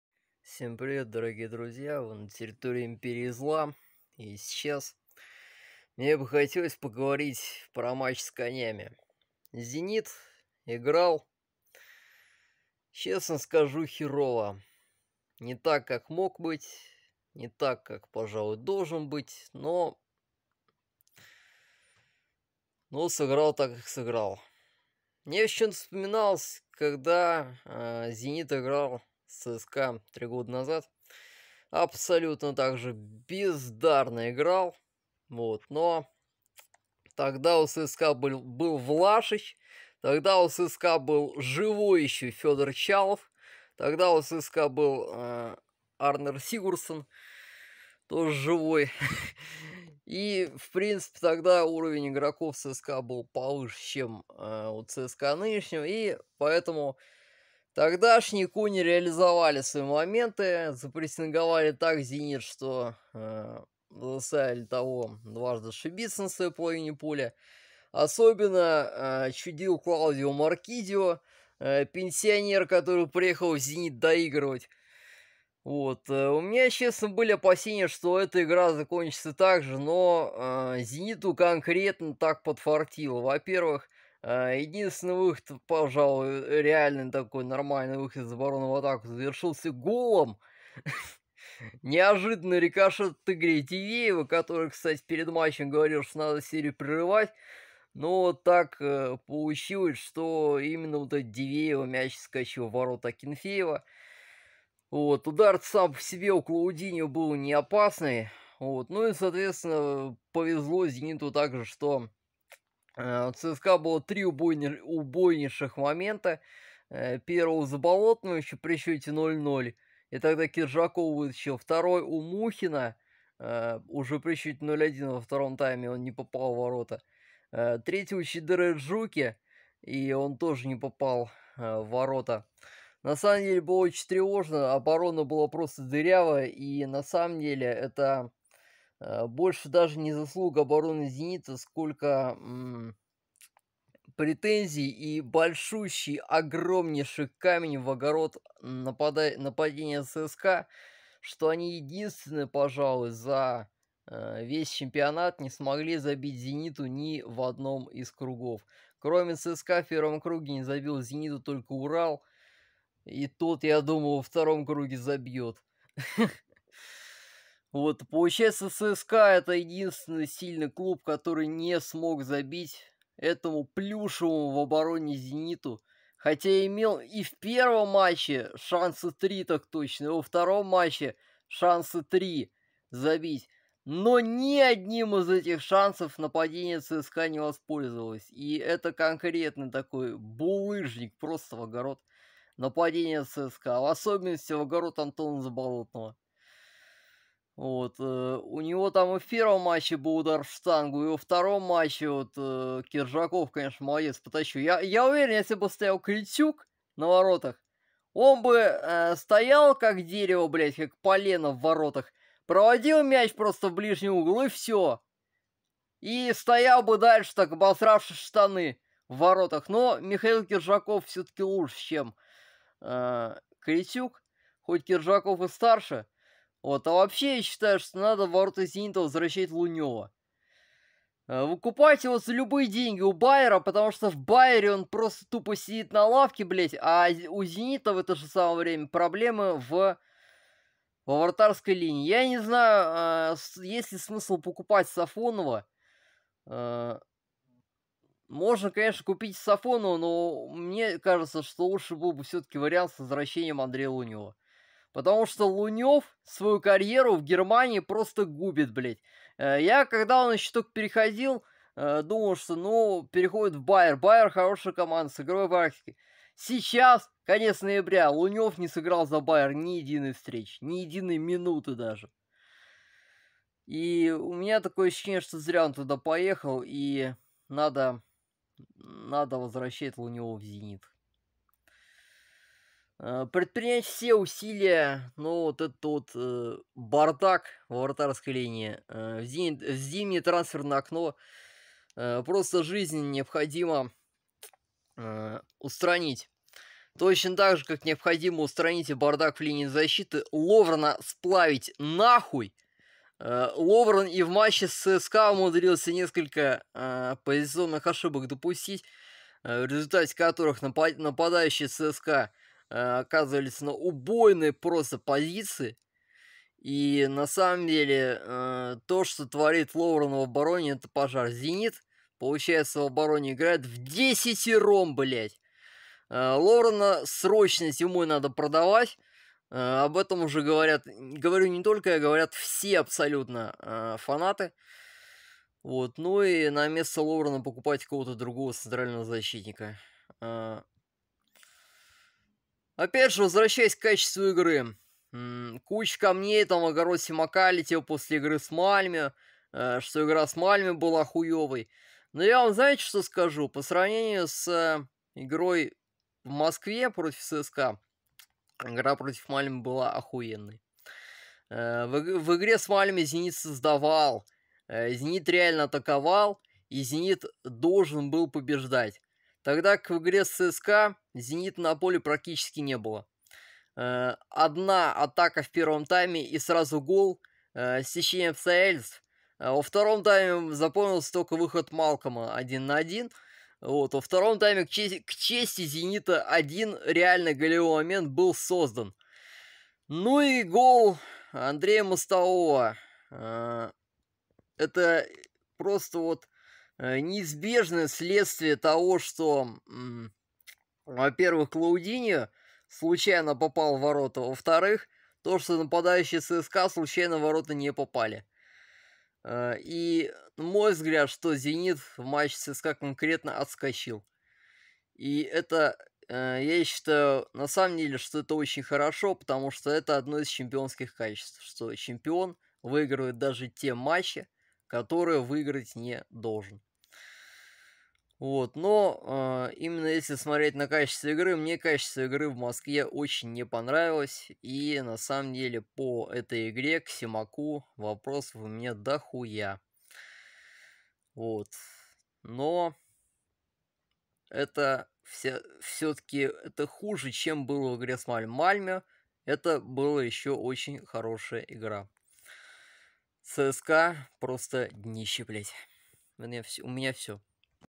Итоги матча с ЦСКА. В Мальмё было лучше.
Отскочили.
К Семаку — дофига вопросов.